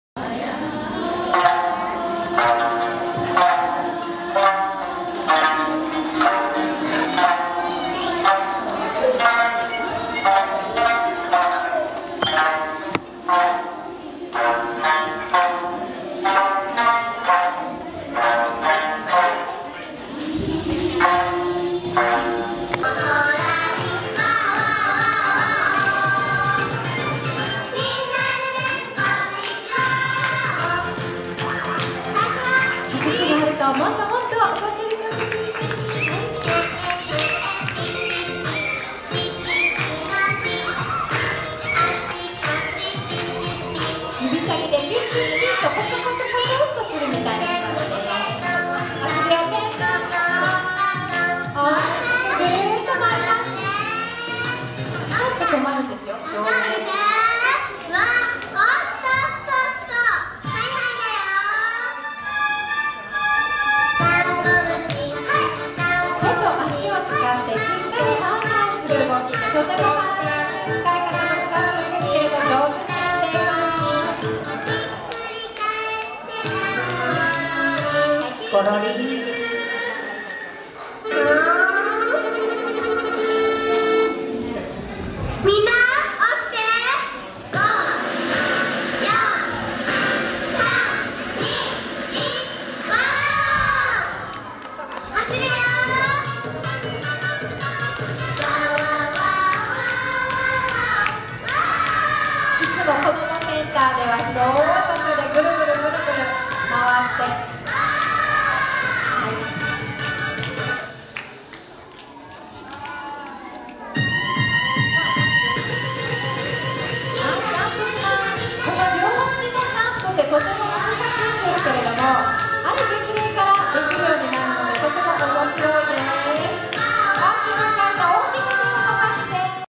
石垣市子どもセンターまつり盛会
2月24日午後2時から石垣市健康福祉センター健診ホールで石垣市子どもセンターまつりが開催され、センターに通う子ども達が舞台を展開。多くの親子連れが参集して、子ども達の演技や演奏などの舞台を鑑賞して、たのしい時間を過ごしていた。
　この日は、宮古島から下地児童館長の来間優美子さんと上野児童館長の上地常美さんが参加。石垣こどもセンターの山里悦子さんとともにサキシマミーティングと題して、宮古島の児童館の様子について語り合う時間を設けていた。
　そのほか、サンシンの舞台やファッションショウ的な舞台、幼児と母親がいっしょに舞台で音楽に合わせてスキンシップしながら遊ぶなど、様々な演目を見せて、会場を沸かせていた。また、この日はデーサービスハピネス平真から15名が車いすに乗って来場して、子ども達の舞台を楽しんでいた。